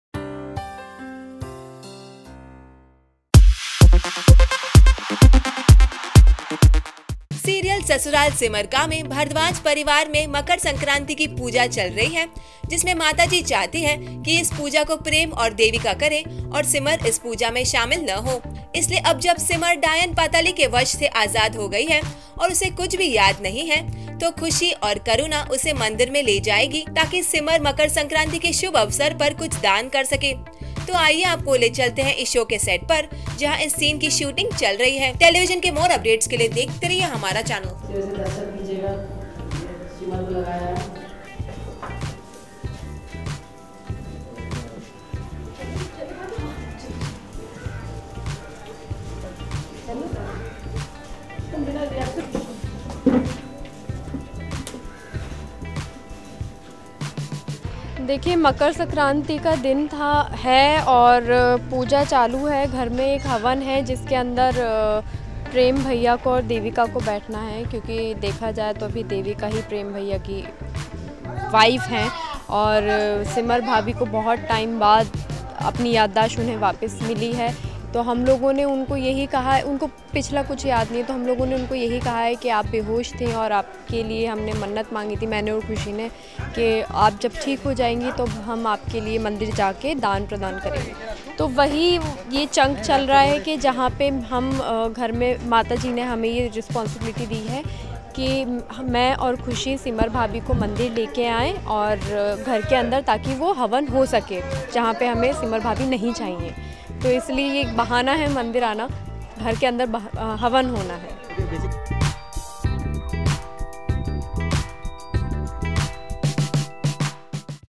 Sasural Simar Ka | Simar To Do Makar Sankranti Puja Without Bharadwaj Family
Serial Sasural Simar Ka mei, Bharadwaj Parivaar mei Makarsankranti ki puja chal rahi hai jismei Mataji chahti hai ki is puja ko Prem aur Devika karein aur Simar is puja mei shaamil na ho. Isiliye ab jab Simar Daayan Patali ke vash mei se azaad ho gayi hai, aur usse kuch bhi yaad nahi hai toh Khushi aur Karuna usse mandir mei le jaaegi taaki Simar Makarsankranti ke shubh avsar par kuch daan kar sake. Toh aaiye aapko le chalte hai is show ke set par jahan is scene ki shooting chal rahi hai. Telivision ke more updates ke liye dekhte rahiye humara channel.